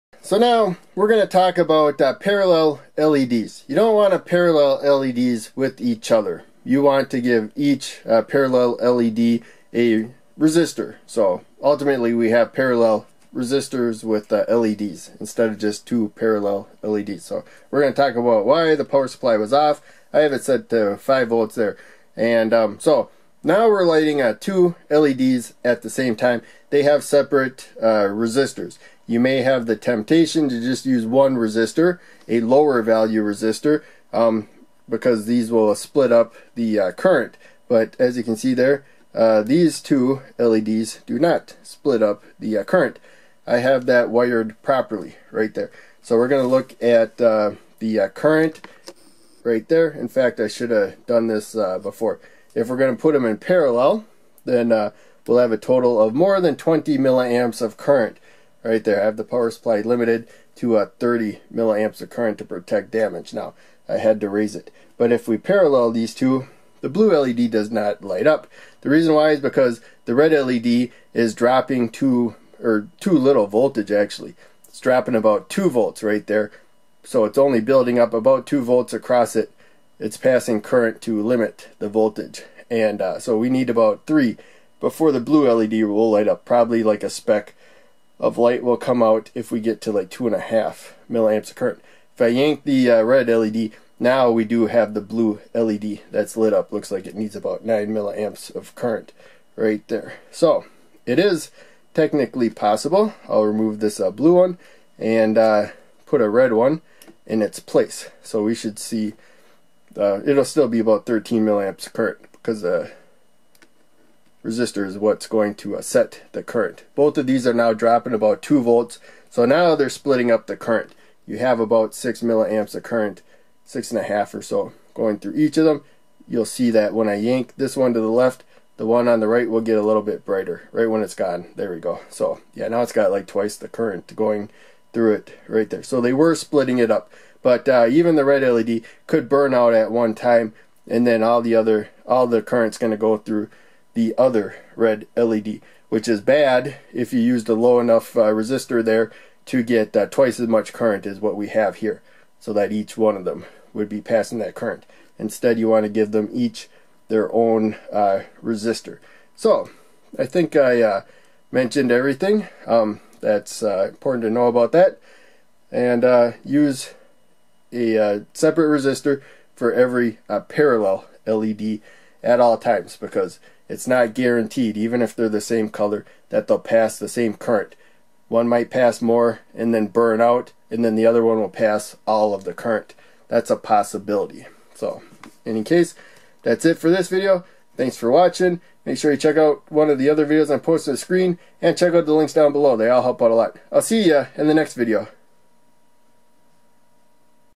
Why each parallel LED should have it's own protective resistor
Parallel LEDs will likely not split current equally. In fact there's a good chance that one LED will pass all the current, while another doesn't pass any. So it's best to give each parallel LED it's own series resistor.